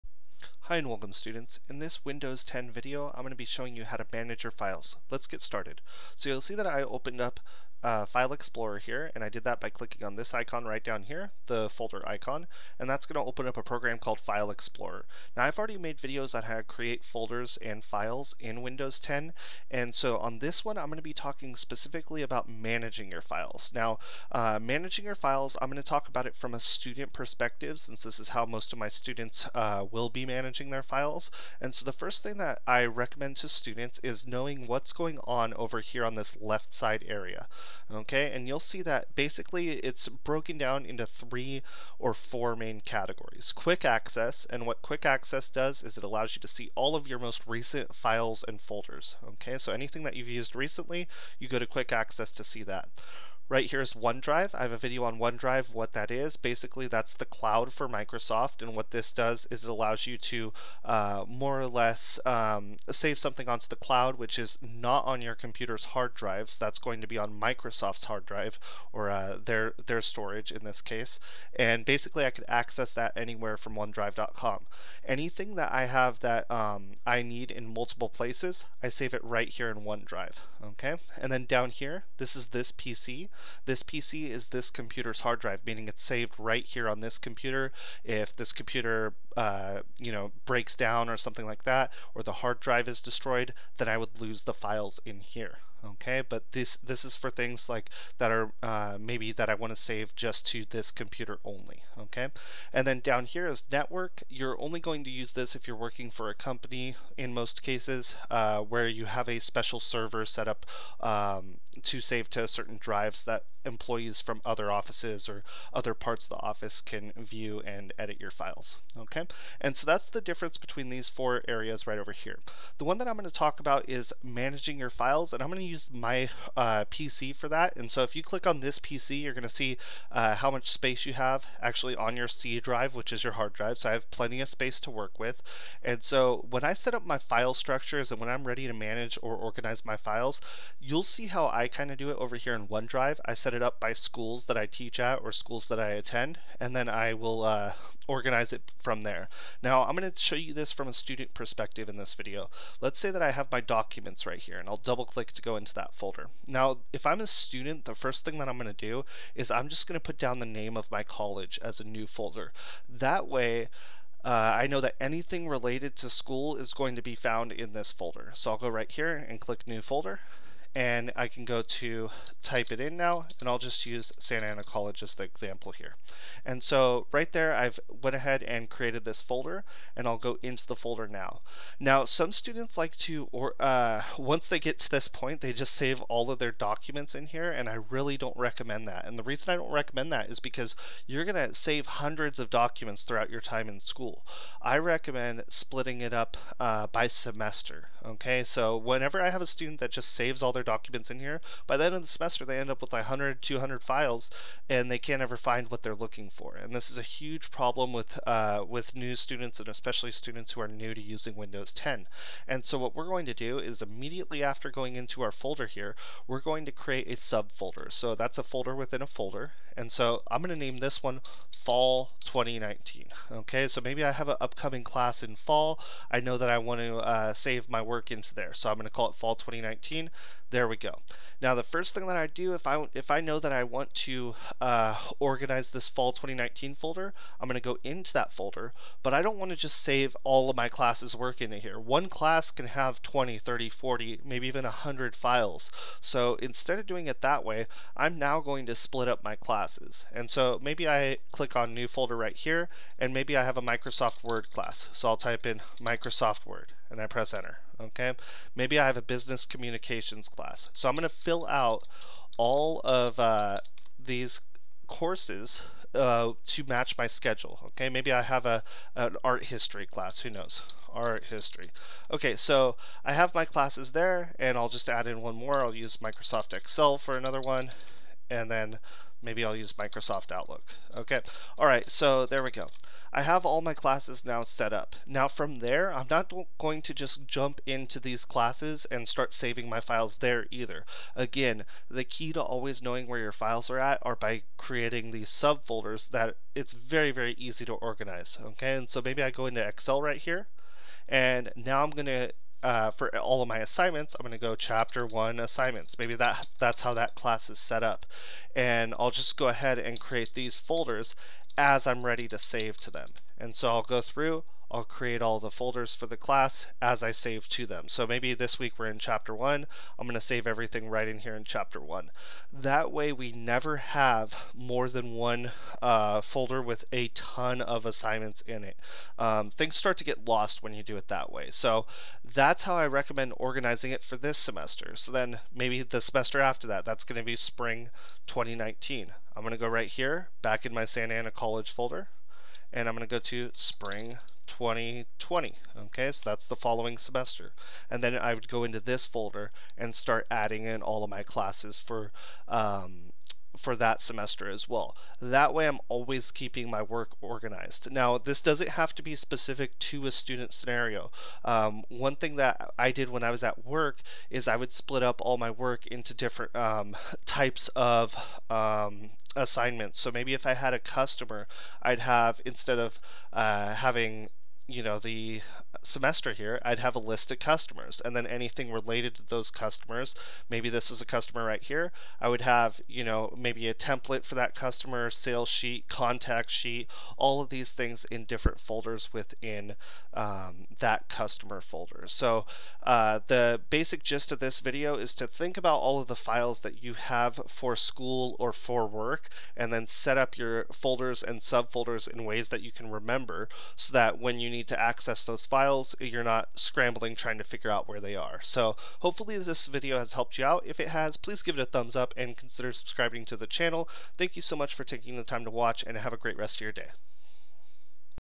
Windows 10 - File Explorer Management Tutorial - How to Organize Files and Folders - Folder Manager
This video covers managing files in file explorer. I've covered how to create and organize files in the past, but this one goes into details about the best way to manage your files at work or at school. We discuss the strategy behind finding your files.